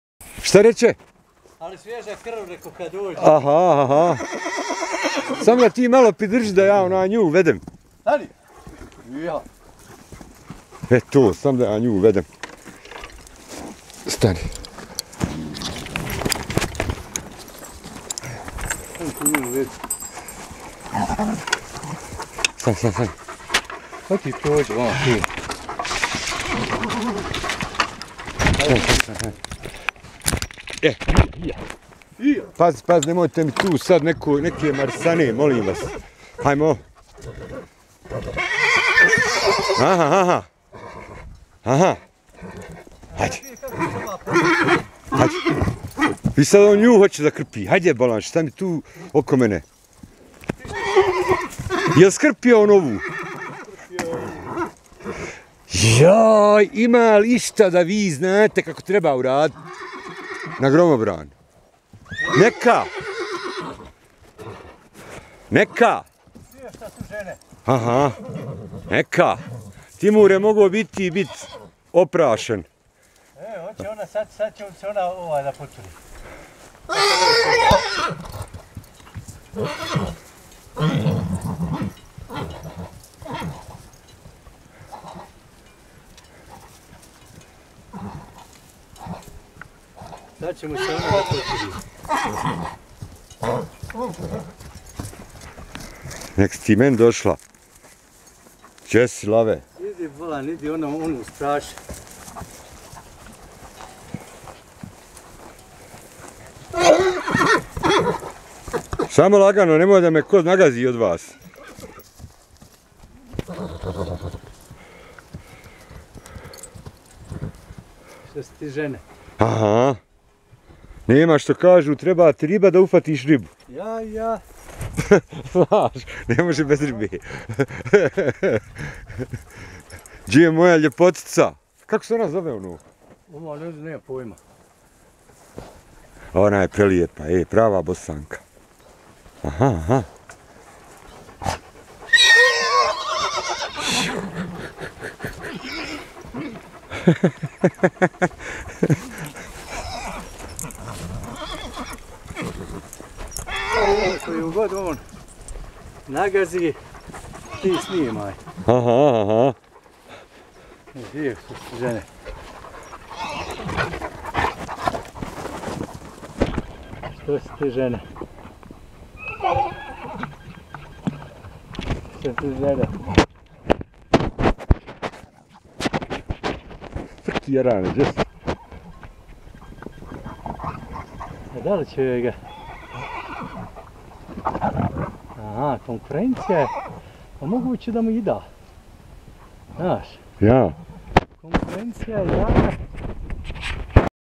Horse love
One stallion and two mares, a recipe for trouble.